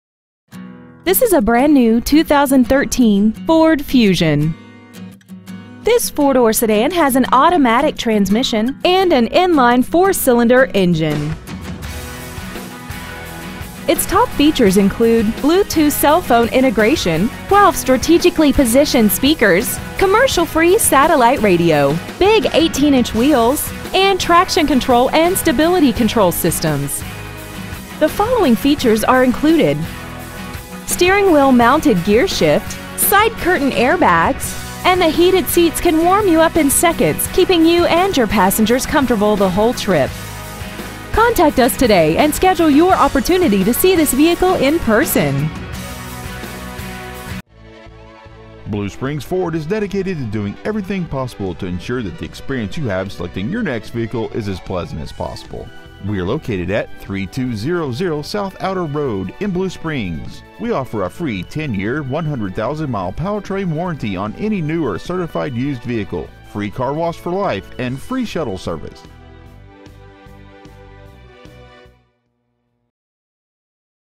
2013 FORD FUSION Blue Springs MO
Year : 2013
 Make : FORD
 Model : FUSION
 Engine : 2.0L I4 16V GDI DOHC TURBO
 Trans . : 6-SPEED AUTOMATIC
 Exterior : RUBY RED
 Miles : 3
 Interior : CHARCOAL BLACK
 Stock : 13159